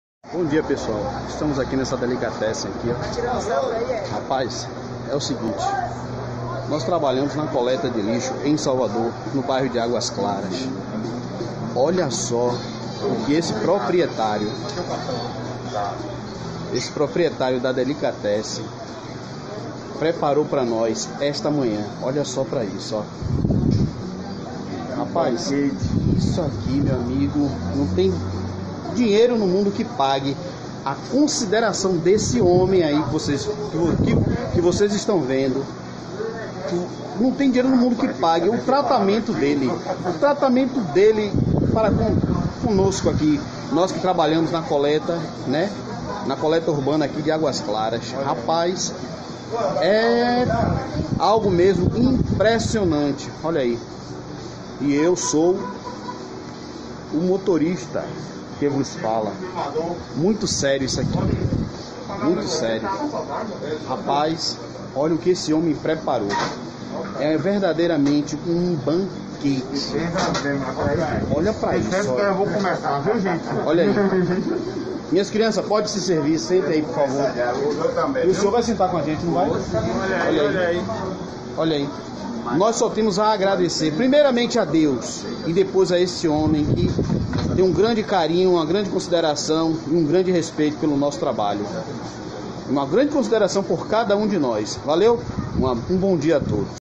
DONO DE PADARIA PREPARA BANQUETE SURPRESA PARA O PESSOAL DA COLETA DE LIXO EM SALVADOR! VEJAM
Amigos do Natureza em ação.
Vejam neste vídeo.
Dono de padaria az banquete surpresa para o pessoal da coleta de lixo em Salvador.. Confiram.
Abraços.